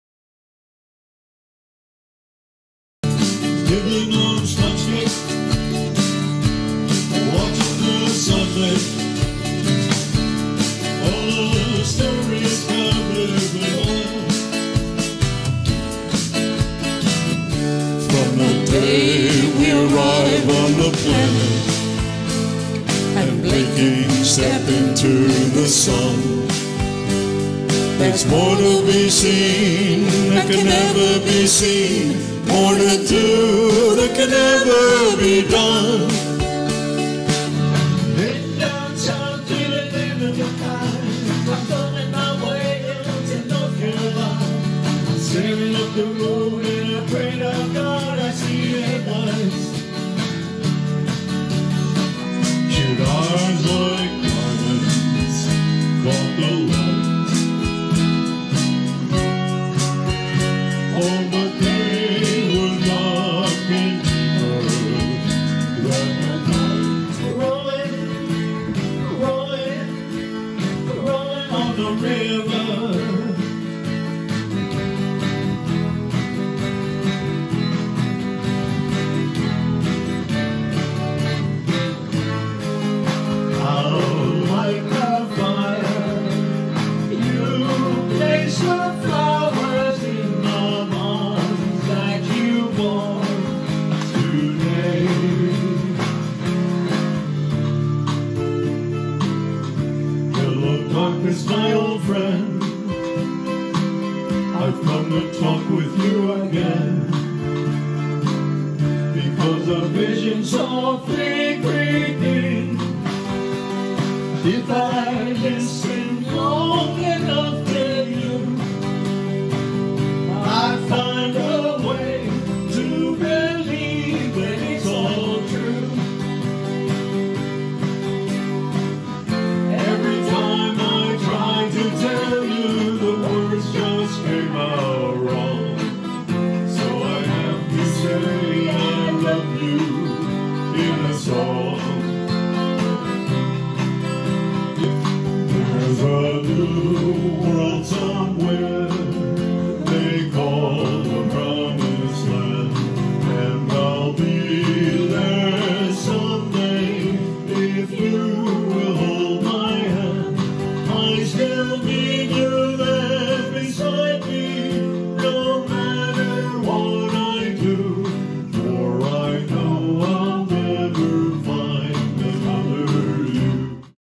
The Stay At Home Band Demo HD 1080p
Here is a group of 10 second samples of some of The Stay At Home Band's live music at a recent South Wayne PorchFest.